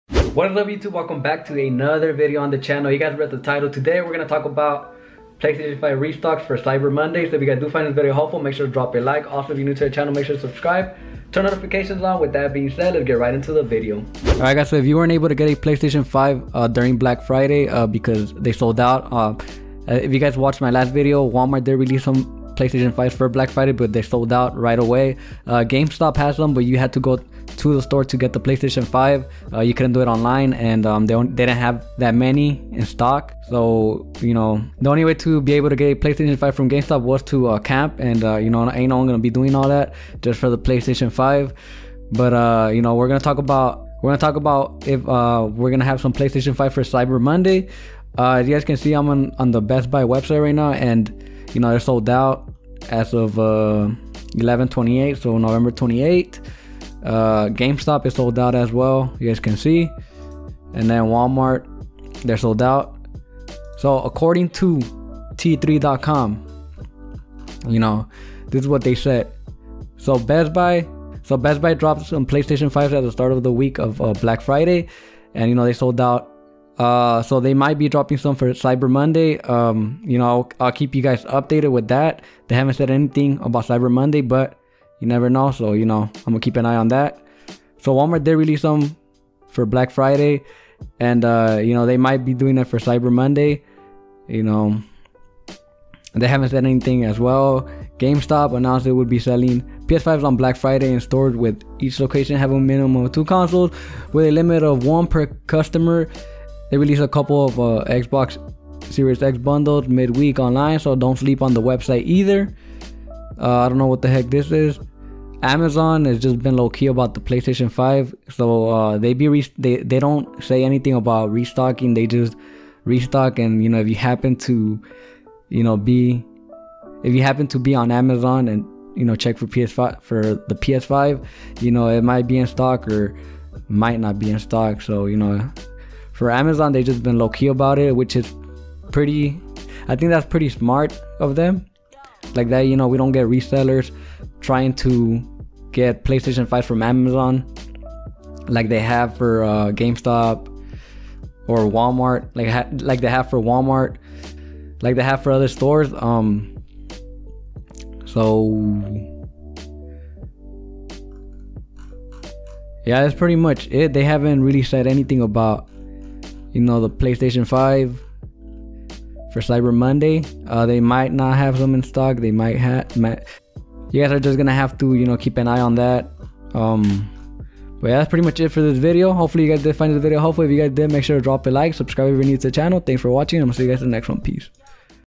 Ps5 Restocking For Cyber Monday?! Ps5 Restock Update!
In this video I Talk About Whether Ps5 Will Restock For Cyber Monday And How To Get A Ps5 After Launch Day!